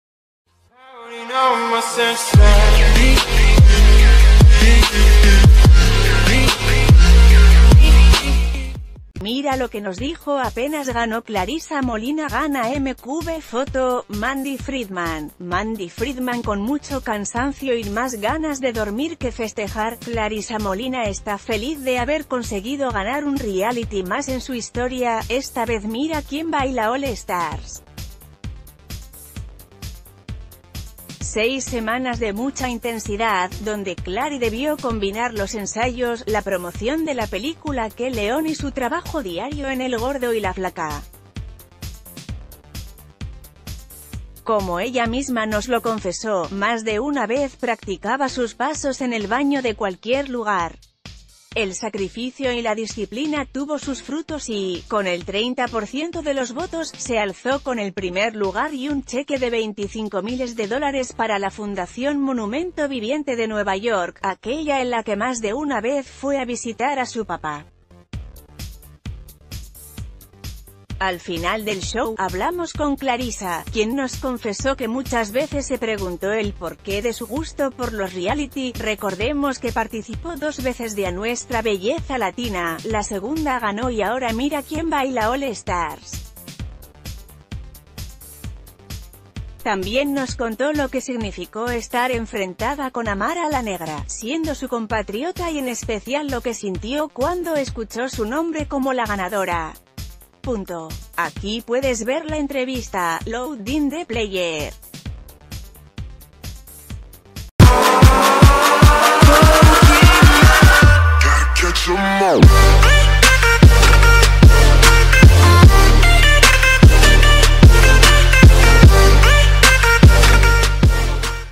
Clarissa Molina después de ganar ‘Mira Quién Baila’: “Nunca esperé que me nombraran”
Mira lo que nos dijo apenas ganó Clarissa Molina gana MQB Foto: Mandy Fridmann / Mandy Fridmann Con mucho cansancio y más ganas de dormir que festejar, Clarissa Molina está feliz de haber conseguido ganar un reality más en su historia, esta vez ‘Mira Quién Baila All Stars’. Seis semanas de mucha int...